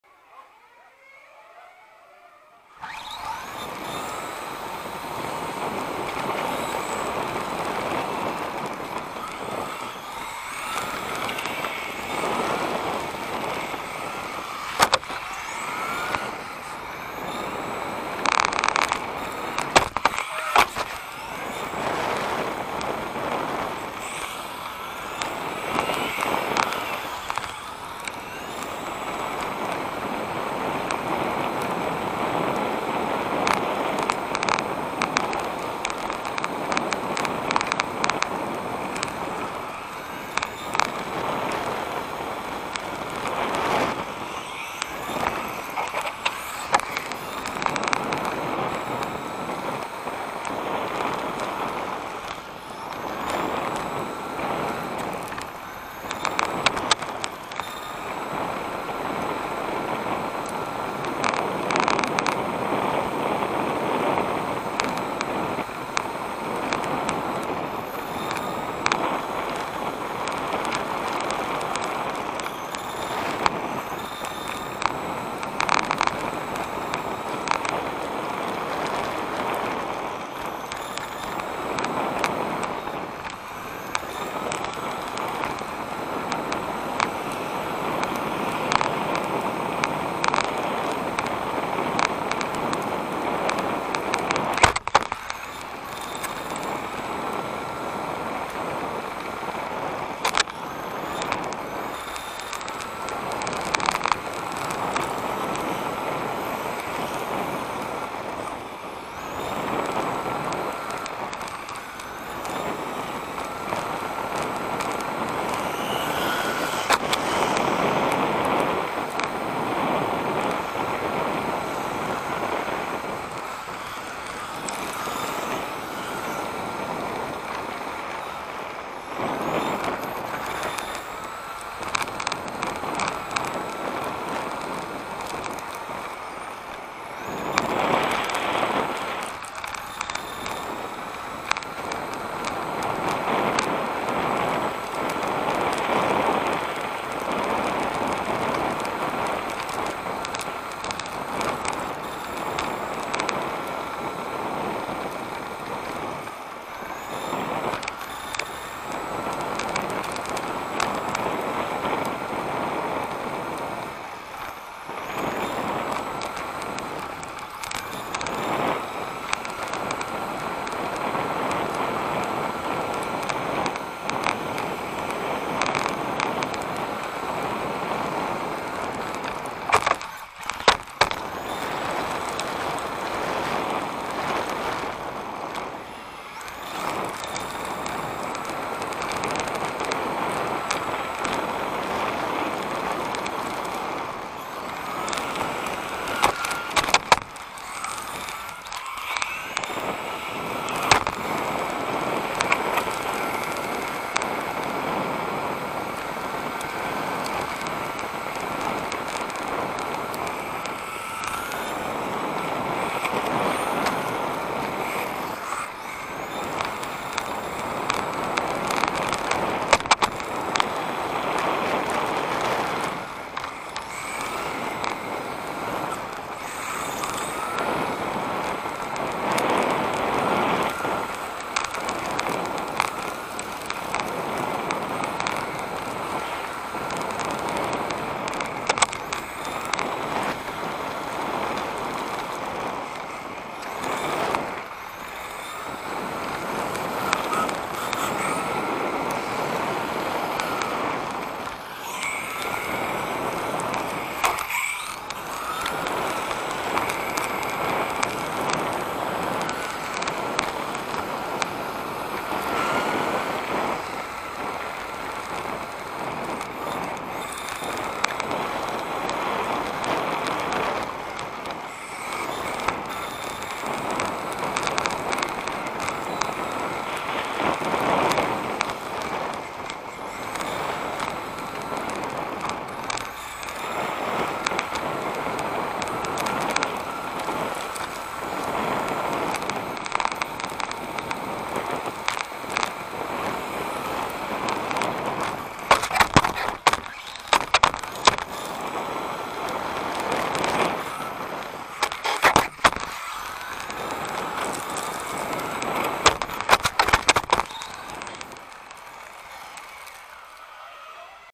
on board video: TSWGT @TRC Round? 20191227
the REAL race
the REAL impact
fight for the glory

my setup
motor: hw V10 gen 1 21.5t
esc: Turnigy Trackstar 80a sensored
servo: TrackStar TS-D99X
receiver: ko propr KR-413FH w/ ko propr EX-II RR
battery: GAONENG 5000mah short pack
gear ratio: 64p 97/36  5.85:1
tires: solars S-T36JGM4W
body: hpi lotus sport elise

TSWGT rules ver1.5 [20191227]
1.only 1/10 touring chassis was allowed (any drive train)
2:all competition, LMP style or copy body shell are not allowed
3:wings
     lexan wing：the maximum height of  the wing is 10mm. the wing must not wider then the ebody
     plastic wing：only the wing comes with the body shell was allowed
4:motor
     brushless：21.5 blinky mode
     brushed：RS-540J / RS-540 Torque Tuned
5:FDR : 5.0 or higher
6:form tires was not allowed